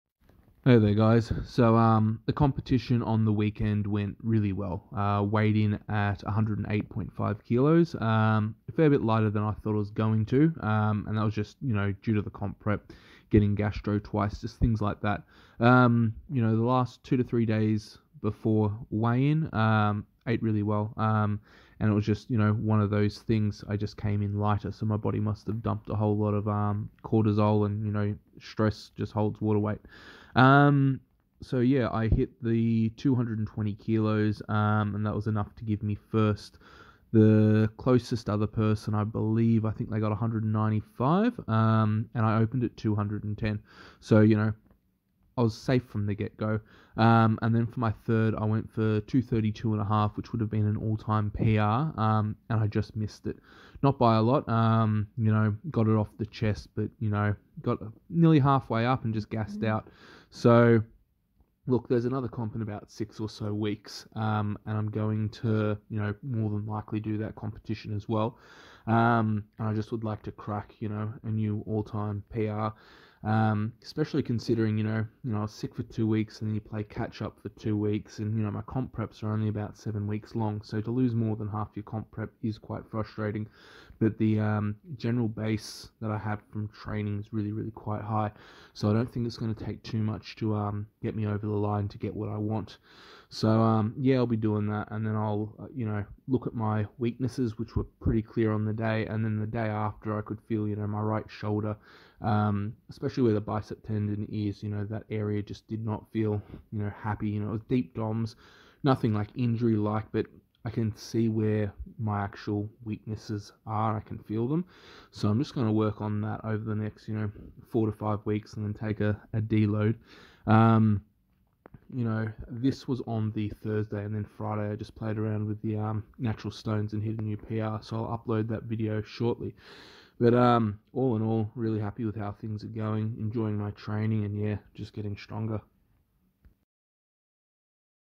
Powerlifting Comp First Place - Bench Press Only Proraw 2023
Had a great comp. When I say the comp on the weekend I was meant to say Thursday. The comp goes across 4 days being Thursday - Sunday.